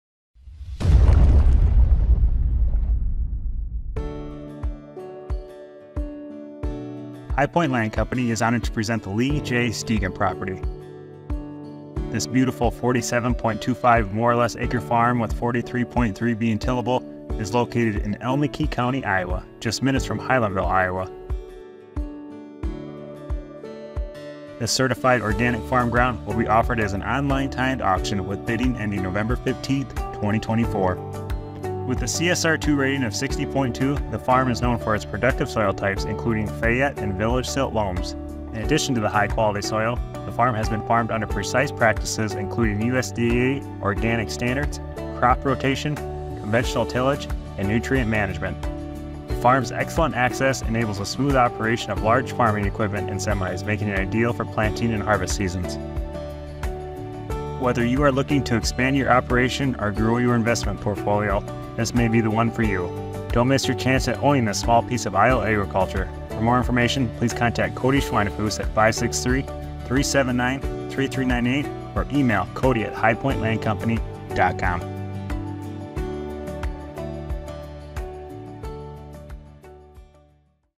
Certified Organic 47-Acre Farm in Iowa for Auction | Bidding Closes Nov 15, 2024
High Point Land Company proudly presents the Lee J. Stean property, a stunning 47.25-acre certified organic farm located in Allamakee County, Iowa, just minutes from Highlandville! With 43.3 acres of tillable land and a CSR2 rating of 60.2, this farm features high-quality soils like Fayette and Village Silt Loams. The property has been meticulously farmed under USDA organic standards, ensuring top-notch soil productivity.

Bidding for this incredible property is open now, with the online timed auction ending on November 15, 2024. Whether you're looking to expand your farming operation or invest in valuable agricultural land, this opportunity is not to be missed.